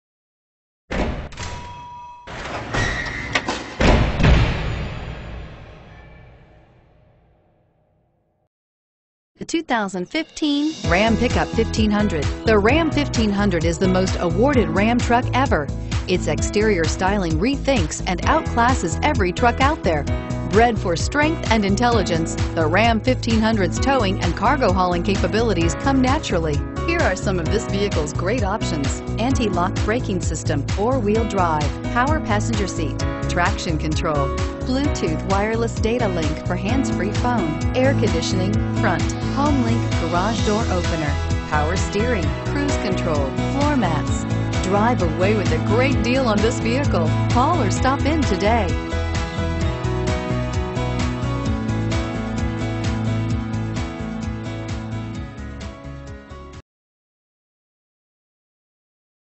2015 Dodge Ram 1500 Gainesville GA G539224
2015 Dodge Ram 1500

4WD. Inherits the road with a sure-footed ease. Put the pedal to the metal because the rubber's on the road and staying there. brbrThis attractive 2015 Dodge Ram 1500 is the rare family vehicle you are trying to find.